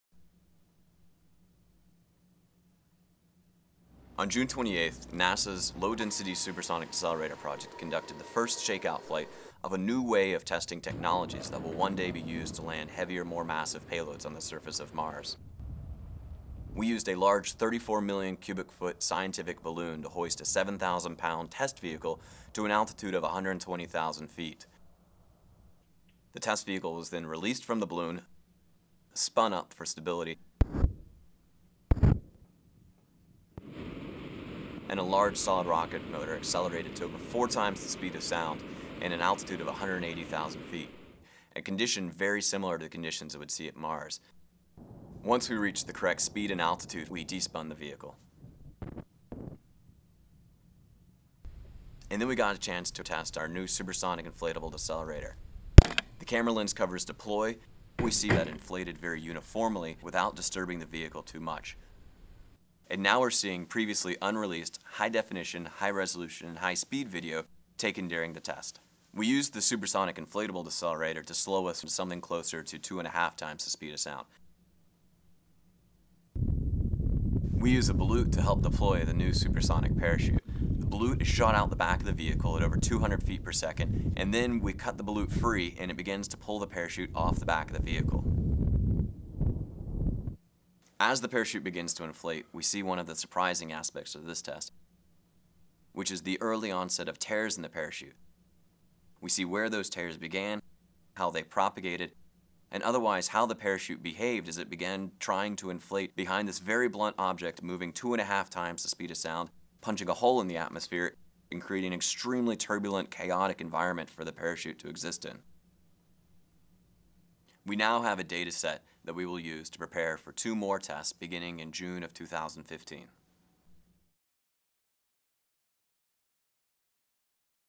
Flying Saucer Test Flight (2014)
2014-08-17: "Ian Clark, Low Density Supersonic Decelerator (LDSD) Principal Investigator, narrates balloon launch, rocket firing and parachute testing on June 28, 2014. The LDSD is a concept for slowing a spacecraft entering Mars' atmosphere at supersonic speeds. For this test, the goal was to slow the test vehicle from four times the speed of sound to 2.5 times the speed of sound."